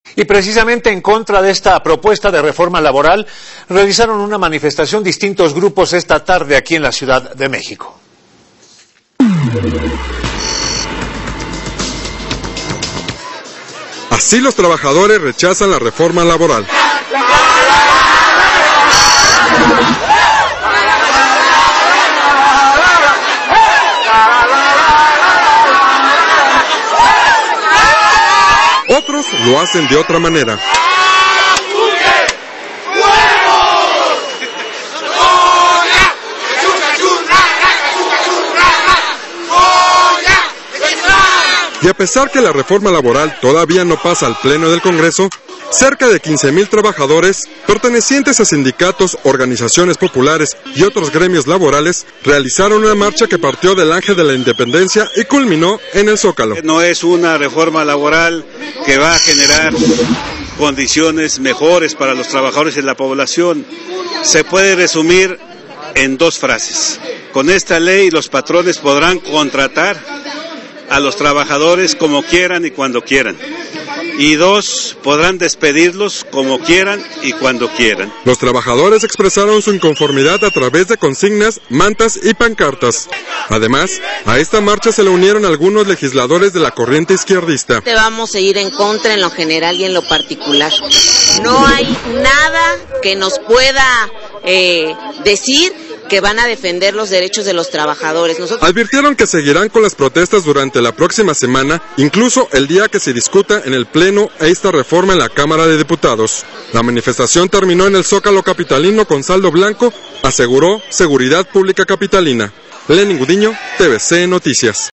Se manifiestan en la Ciudad de México contra la Reforma Laboral
TRABAJADORES SALEN A LAS CALLES DE LA CAPITAL PARA MANIFESTARSE EN CONTRA DE LA REFORMA LABORAL...